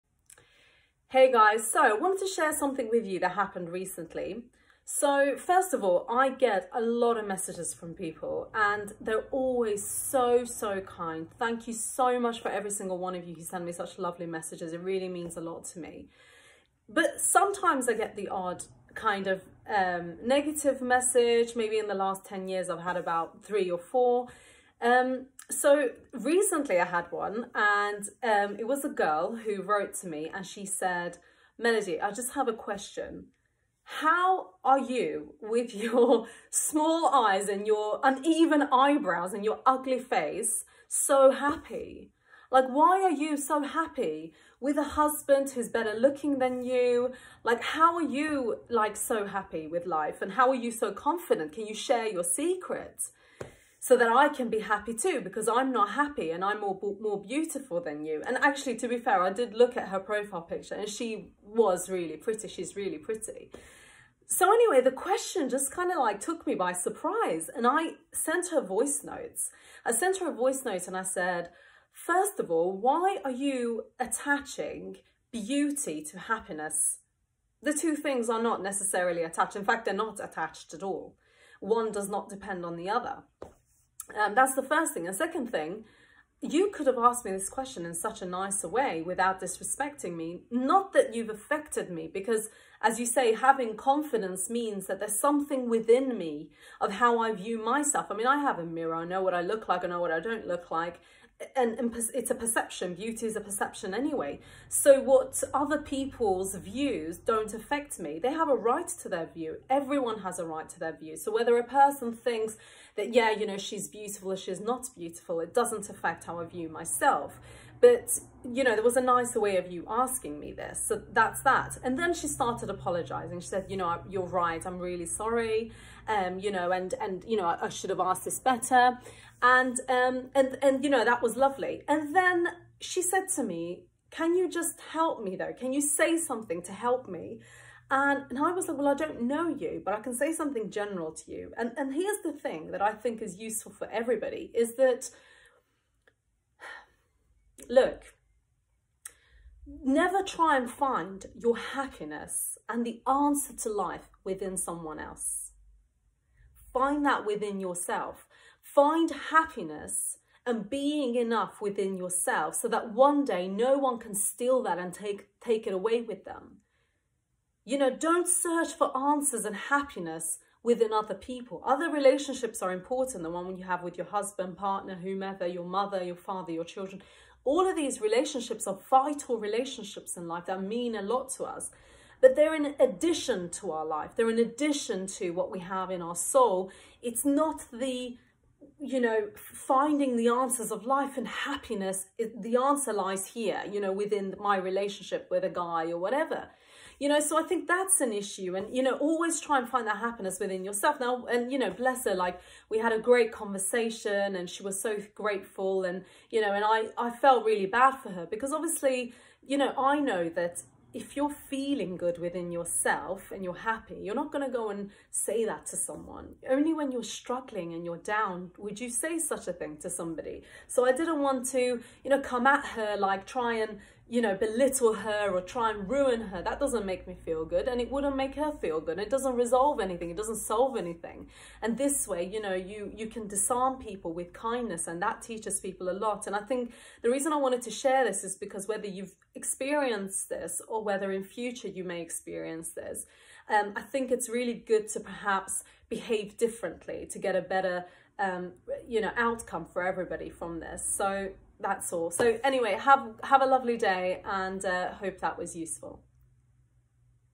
How to deal with negativity.
Recently I received a rare negative message from someone. It really fascinated me about the process of dealing with other people’s negative perceptions. Never try and find validation and happiness in other people- it needs to be owned within you. Hope you guys like the video- share if useful.